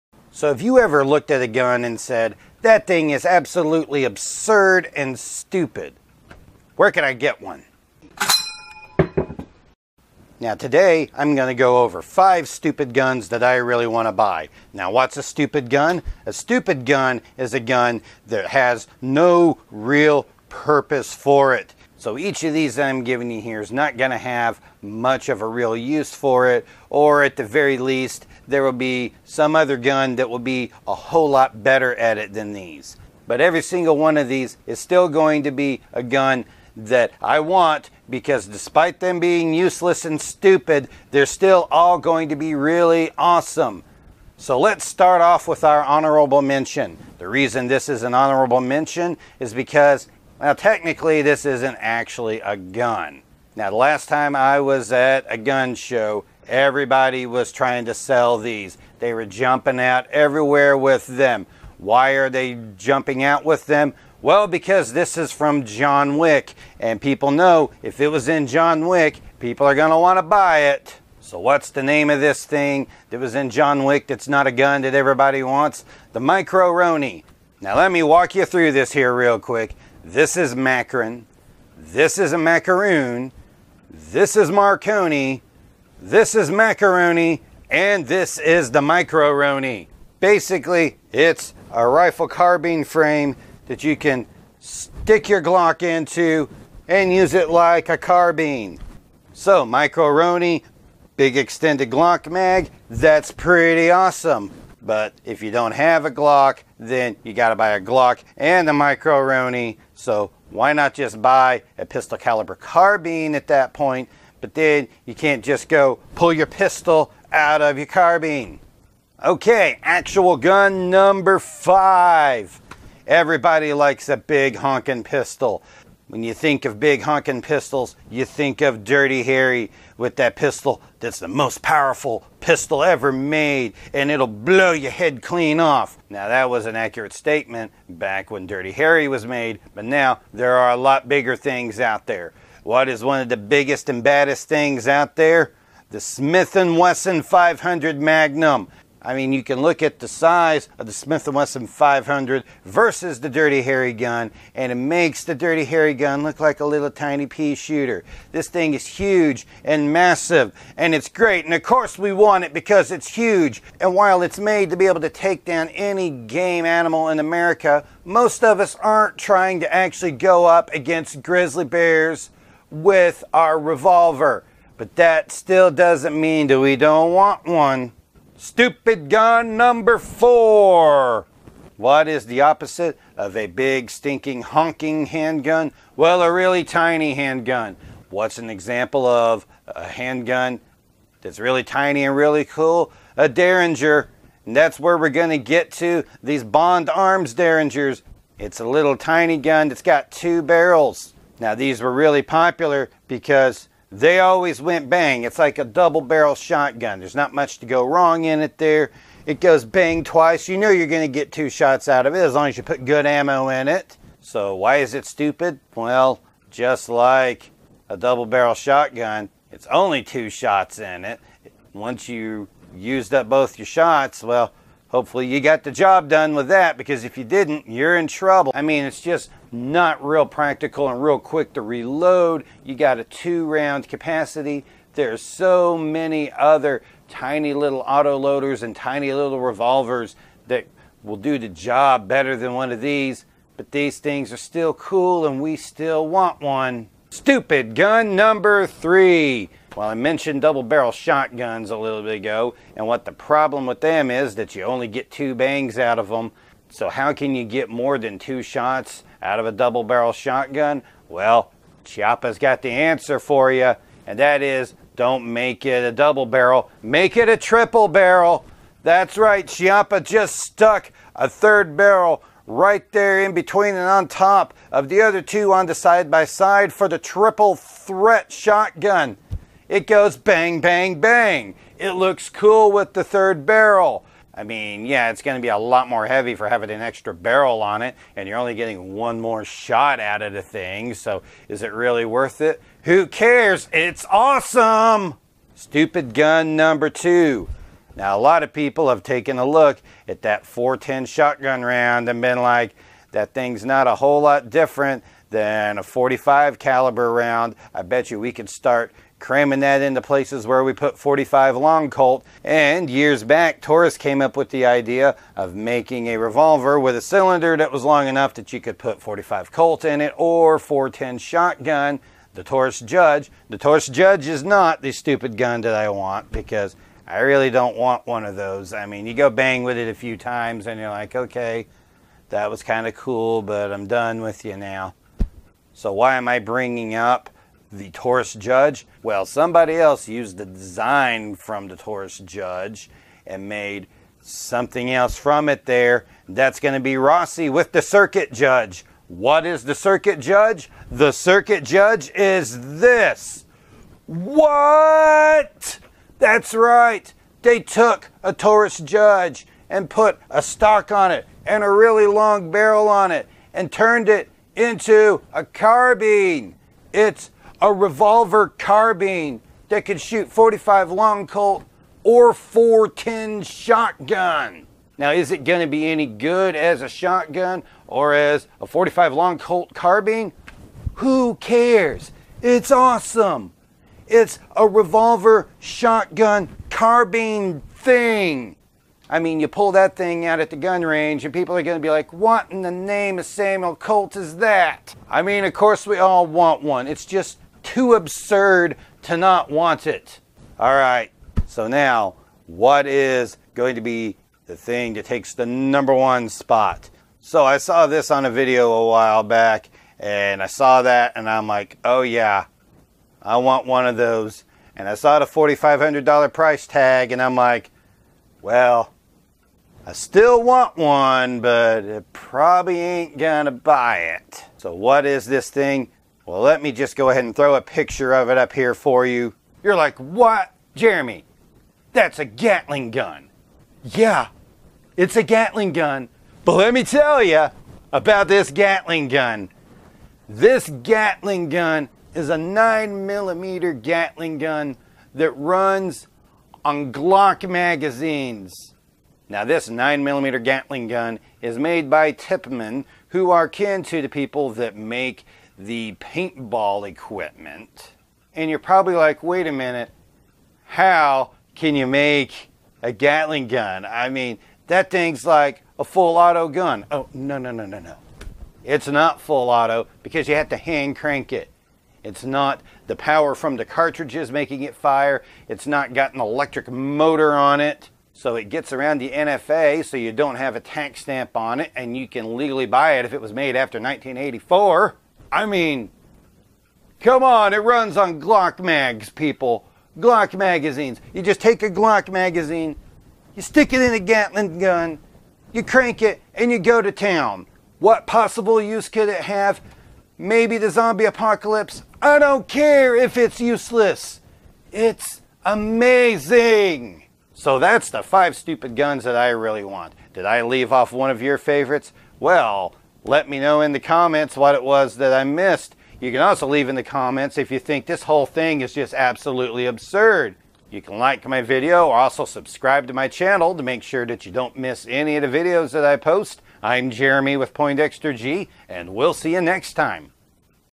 5 Stupid Guns That I Want
These are stupid guns that will have no useful purpose to me. I want them anyway. Take a look at my list and see if you agree!

0:00 Intro
0:44 Honorable Mention
1:58 Number 5
2:58 Number 4
4:06 Number 3
4:55 Number 2
6:40 Number 1

Photo Credits:
Taurus Judge - Photo by Harvey Henkelmann

Micro Roni photos from CAA

Gatling Gun photos from Tippmann Armory and Copper Custom Armament

Circuit Judge photos from Rossi